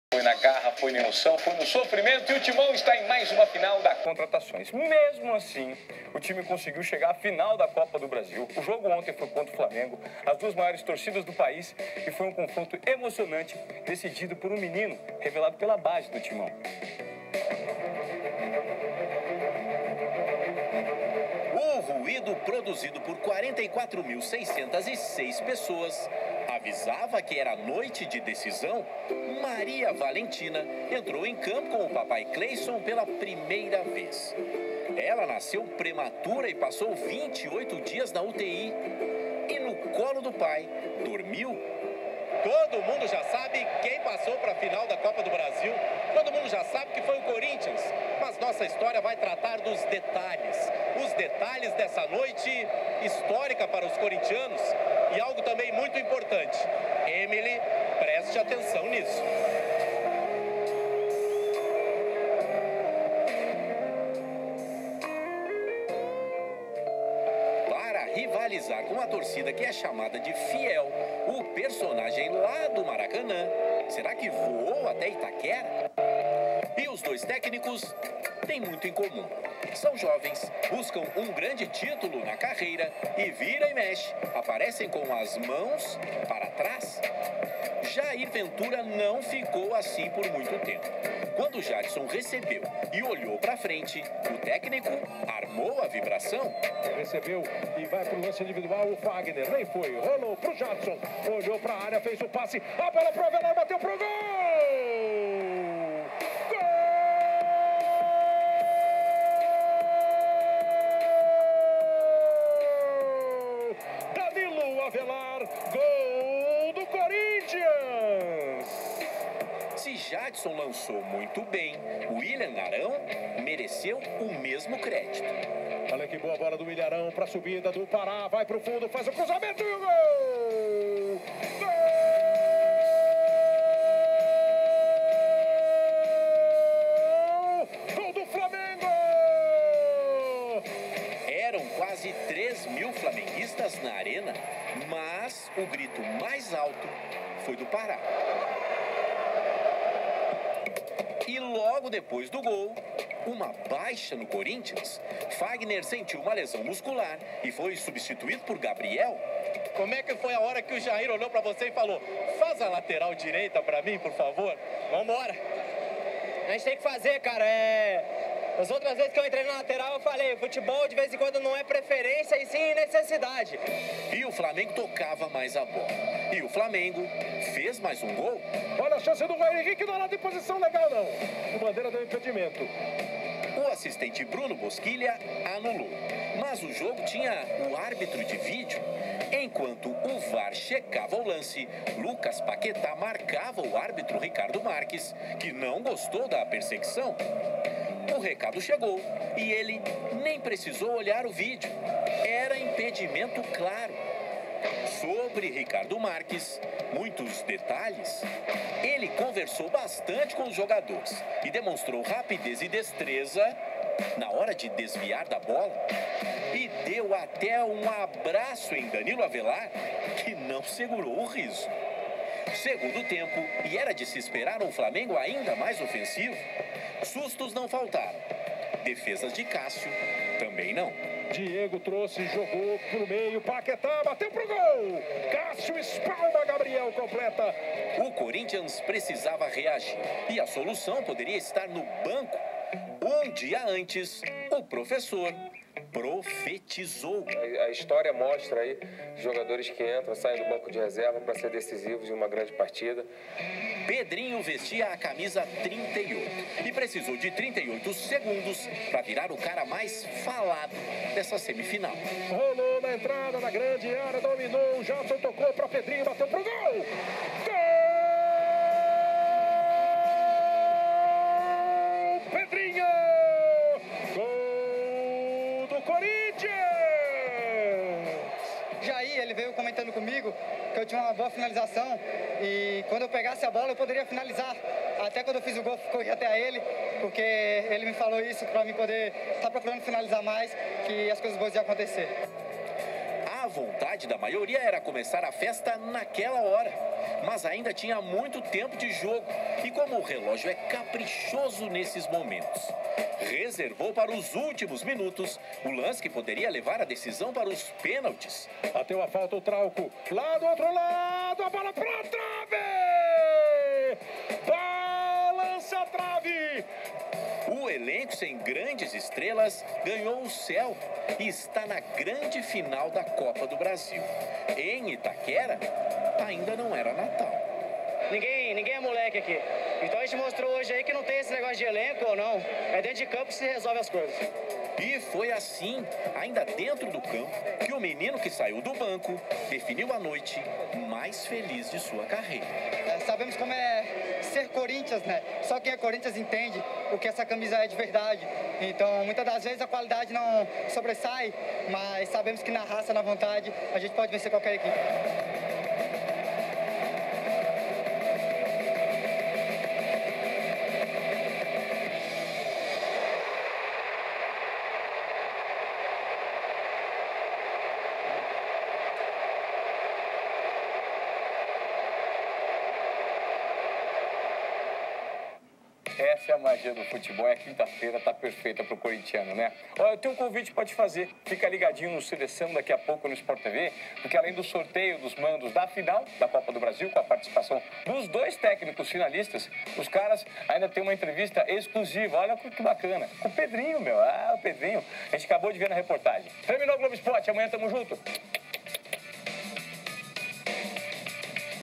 CORINTHIANS (class) 2x1 flam semi copa do br 2018
FICHA TÉCNICA
CORINTHIANS 2X1 flam
Copa do Brasil 2018 semifinal volta
Estádio: Arena Corinthians
Data: 26 de setembro, às 21h45
Árbitro: Ricardo Marques Ribeiro (Fifa/MG), Bruno Boschilia (Fifa/PR) e Bruno Raphael Pires (Fifa/GO)
Público  44.406 presentes (44.249 pagantes 157 nao pag) renda R$ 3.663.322,00 /
Cartão amarelo: Douglas (COR); Lucas Paquetá, Willian Arão e Marlos (FLA)
Cartão vermelho: Não houve.

GOLS: Danilo Avelar (1-0, 13'/1ºT) Henrique (Contra, 1-1, 17’/1ºT) e Pedrinho (2-1, 23'/2ºT)

CORINTHIANS: 12-Cássio; 23-Fagner (5-Gabriel, 22'/2ºT), 14-Léo , 3-Henrique e 35-Danilo Avelar; 15-Ralf, 30-Douglas, 10-Jadson e 22-Mateus Vital (16-Araos, 37'/2ºT); 25-Clayson (38-Pedrinho, 22'/2ºT) e 11-Romero - T: Jair Ventura 27-Walter, 13-Marlon, 34- Pedro Henrique, 2-Mantuan, 33- Carlos Augusto, 17-Thiaguinho, 20-Danilo, 21-Sergio Diaz, 47-Emerson
flam: 1D Alves; 21Pará, 43Léo, 15Réver e 13Trauco; 8Cuéllar, 5W Arão (29Lincoln, 28'/2ºT) e 10Diego (14Vitinho, 18'/2ºT);  11Paquetá, 7E Ribeiro e 19H Dourado (17Marlos, 36'/2ºT)- T: M Barbieri 37Cesar 30Thiago 2Rodinei 44-Rhodolfo, 26Thuller 6Rene 27Romulo 22 Mateus 28Berrio